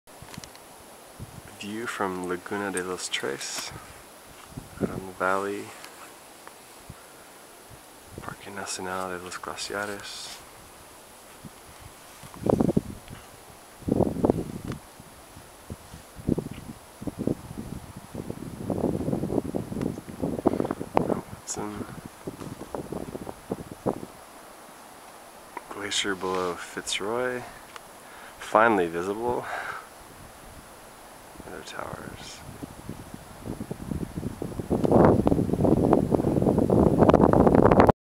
Fitz Roy & Poincenot from Laguna de los Tres
I shot this during a lucky break in the weather this past January. This is the most popular day hike in the El Chalten area, and for good reason. During 3-4hrs of moderate hiking, you cross alpine lakes, turcoise, glacier fed streams and get a series of awesome views of the Fitz Roy range and the Piedras Blancas glacier (among others). Hands down the best day hike I have ever done.